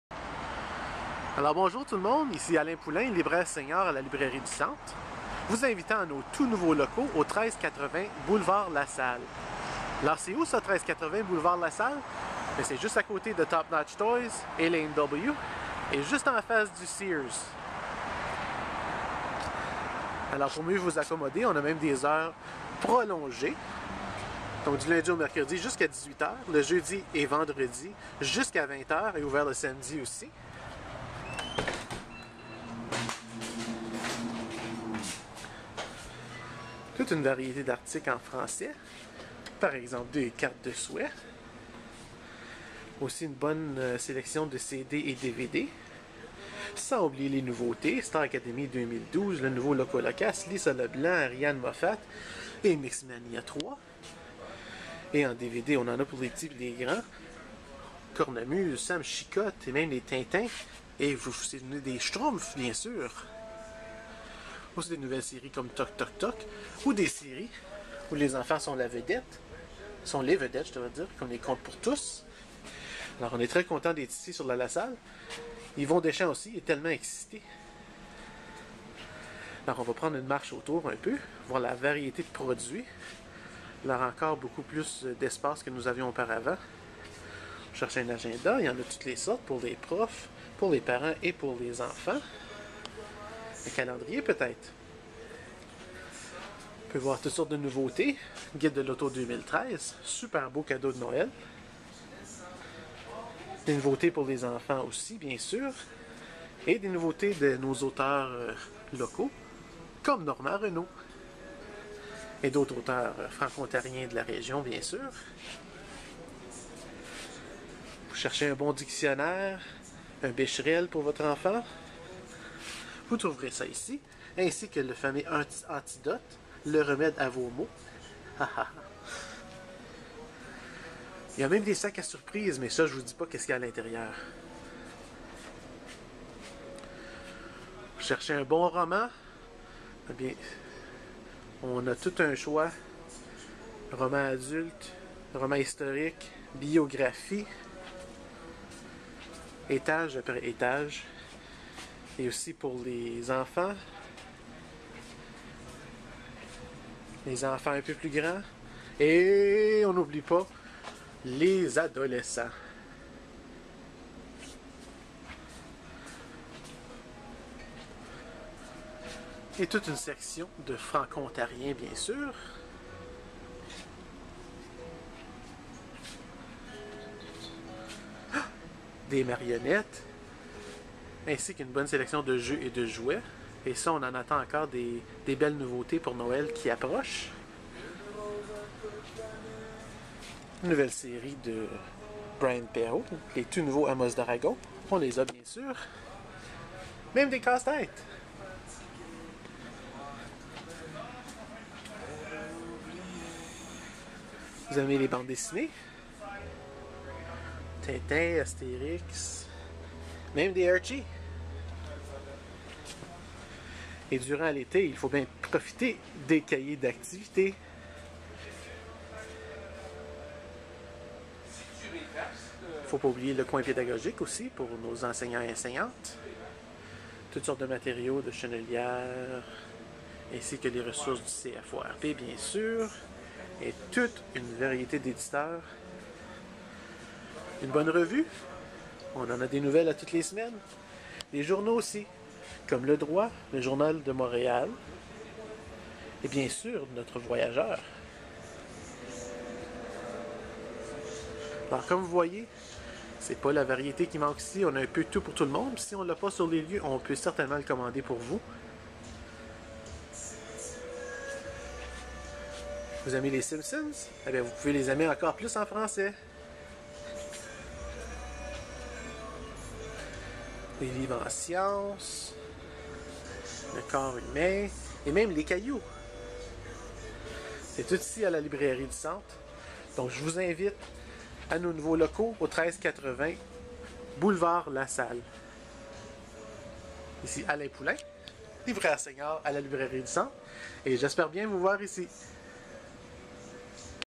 Librairie du Centre Sudbury - nouveaux locaux!
Une petite visite de nos nouveaux locaux sur le Boulevard Lasalle! ***Je ne lisais pas de texte, donc le francais n'est pas parfait Vous remarquerez que je dis ''bien sur'' assez souvent... :-)